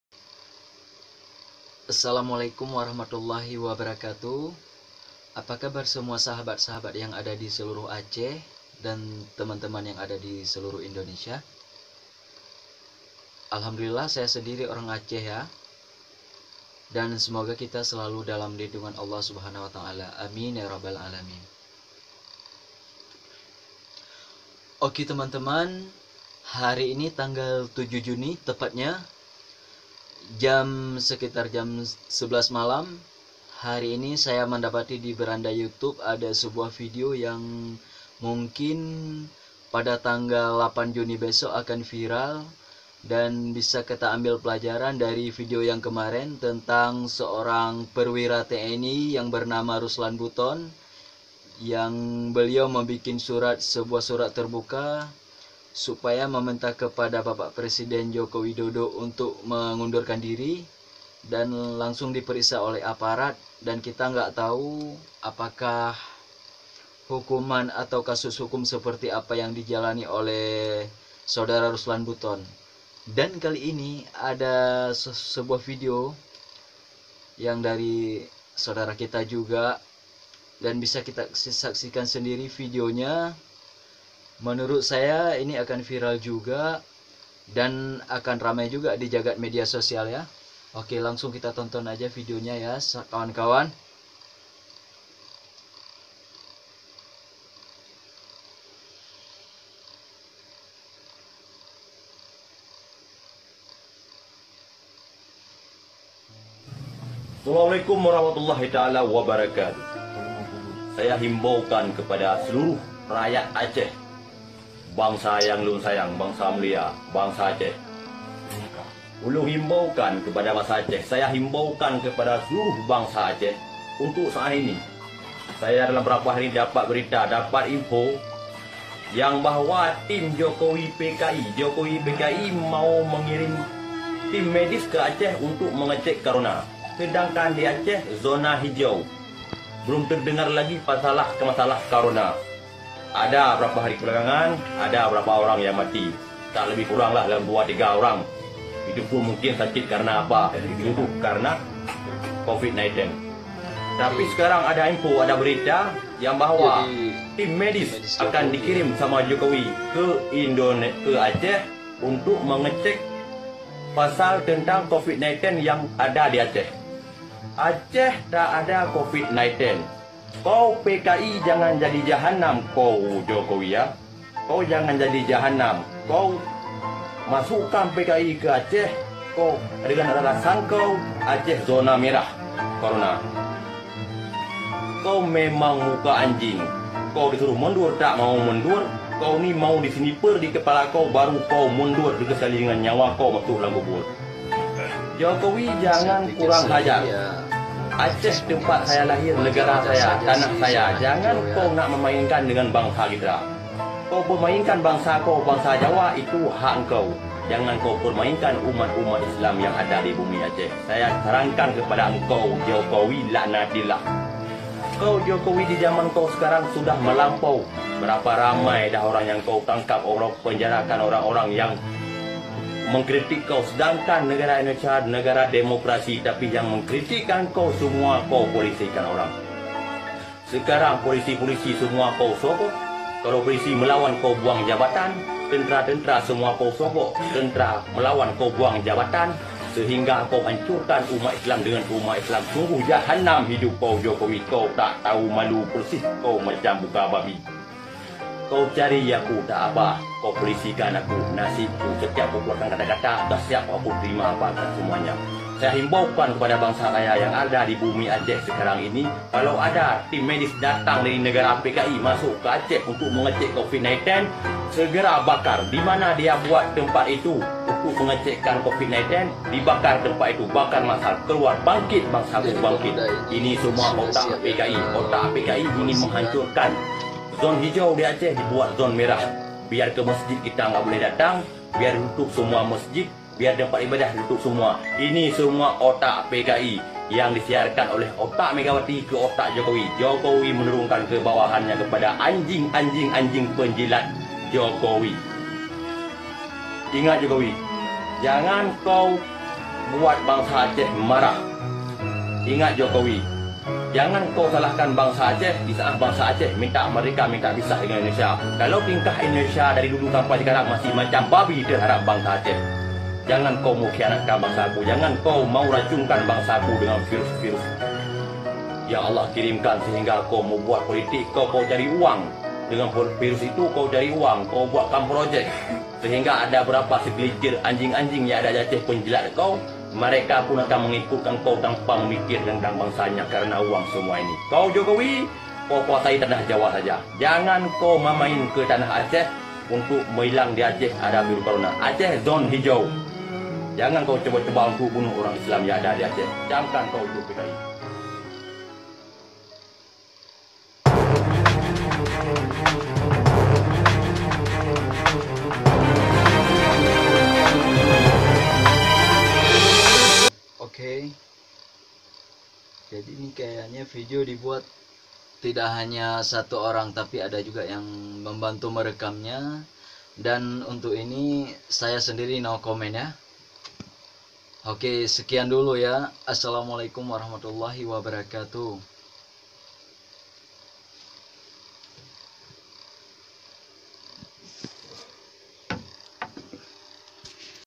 REACTION SAYA ~ PRIA ASAL ACEH INI BERANI HINA JOKOWI
Di vidio kali ini saya akan me REACTION sebuah vidio yang baru~baru ini muncul di beranda youtube dan vidio itu di lontarkan  seorang pria asal ACEH yang menghina jokowi dan sudah viral di berbagai media sosial,,, Ayooo kawan~kawan mari kita dengarkan sama~sama bagaimana vidio komentar dan hinaan seorang pemuda ini terhadap PRESIDEN REPUBLIK INDONESIA SAAT INI, YAITU Mr, JOKOWI DODO...!!!